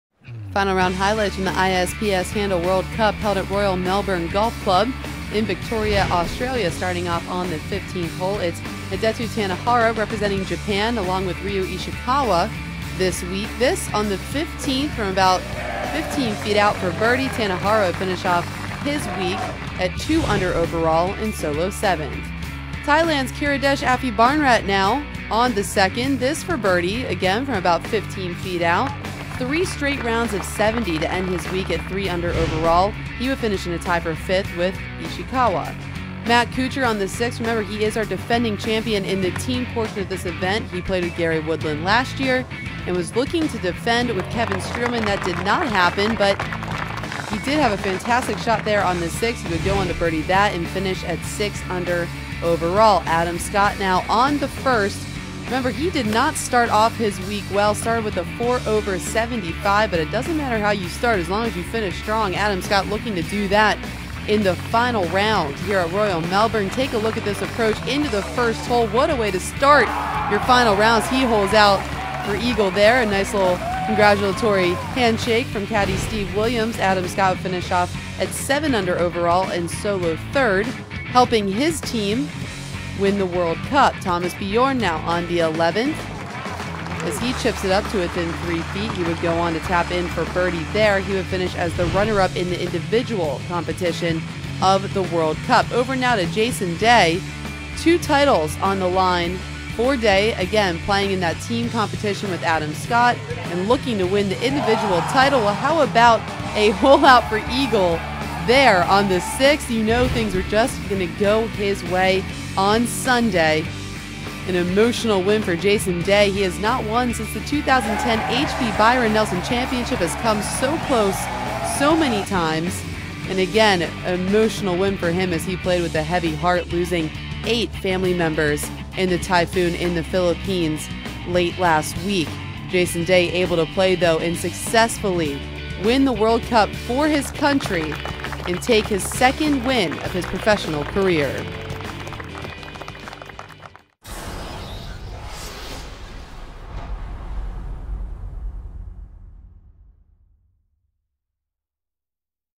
Australia wins the ISPS HANDA World Cup: Round 4 Recap
In the final round of the ISPS HANDA World Cup of Golf in Australia, Jason Day and Adam Scott win the World Cup title for Australia.